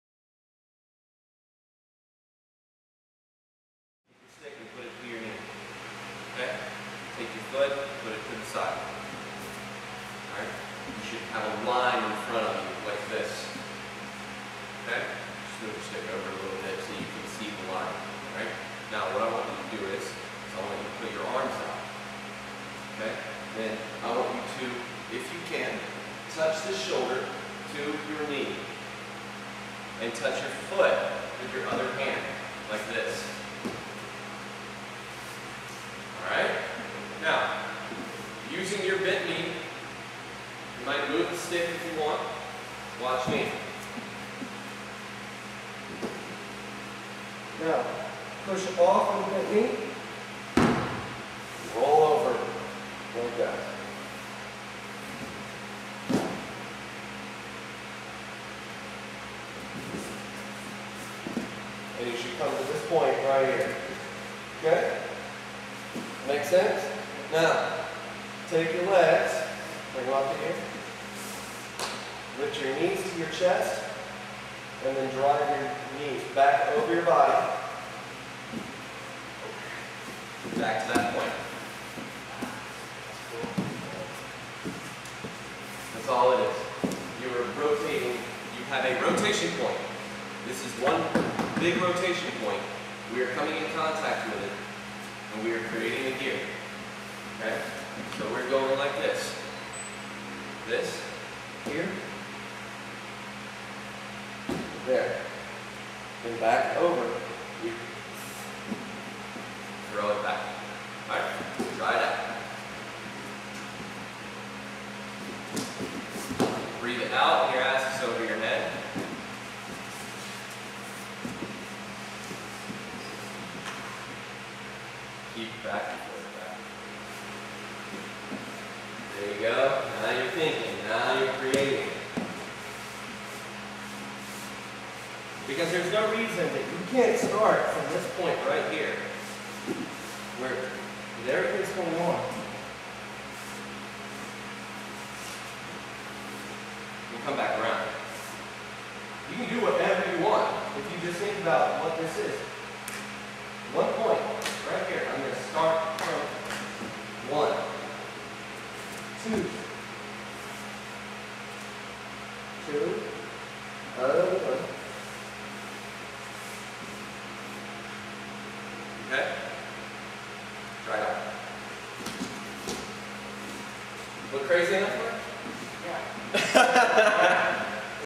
Pramek rolls with Matt Powell
Matt Powell of Pramek demonstrating rolls at Premier Martial Arts.